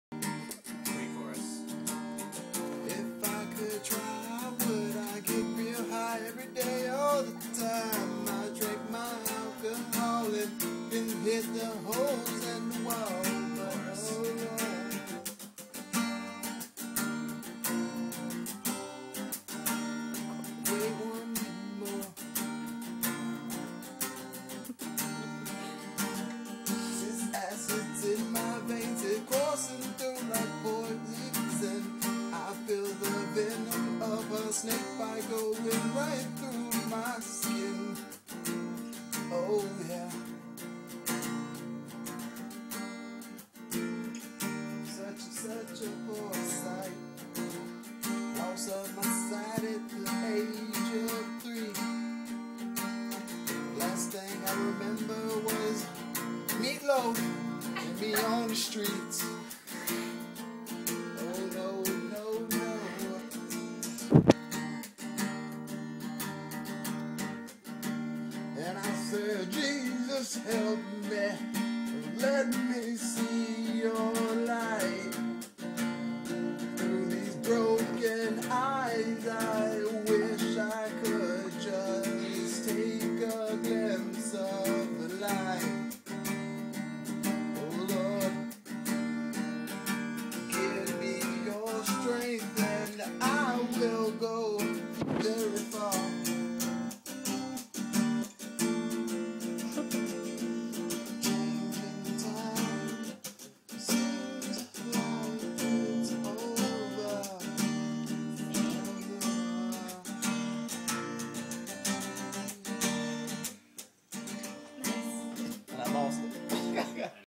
All I remember was, MEATLOAF
Brandonnnn singing an original.